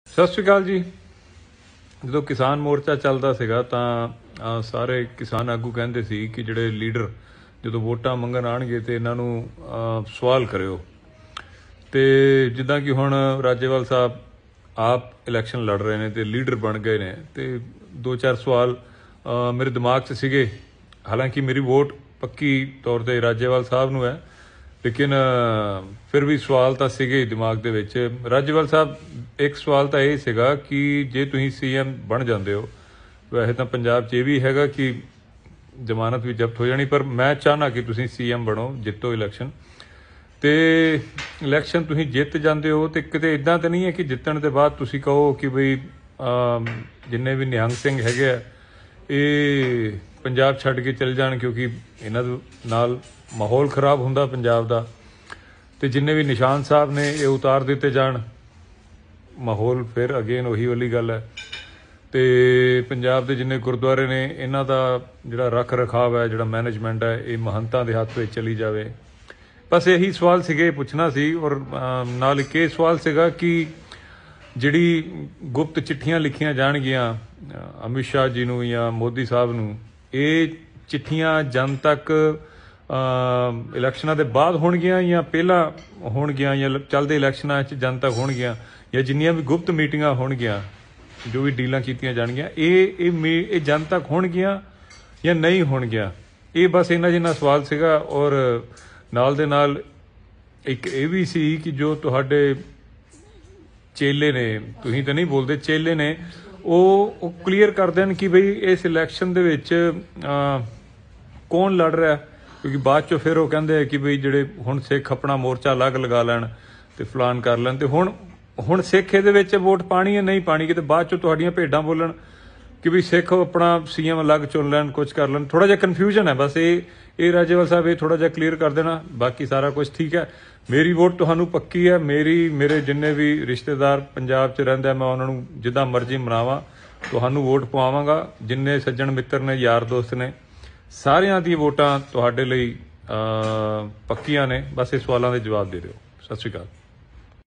Deep sidhu ਦਾ dost ਕਹਿੰਦਾ rajewaal ji ਤੁਹਾਡੇ ਮੁੱਖ ਮੰਤਰੀ ਬਣਨ ਤੋਂ ਬਾਅਦ ਸਿੱਖ ਪੰਜਾਬ ਚ ਰਹਿ ਸਕਦੇ ਆ
ਜੇ ਮੁੱਖ ਮੰਤਰੀ ਬਣ ਗੇ ਤਾਂ ਨਿਹੰਗ ਸਿੰਘ ਪੰਜਾਬ ਚ ਰਹਿ ਸਕਦੇ ਨੇਂ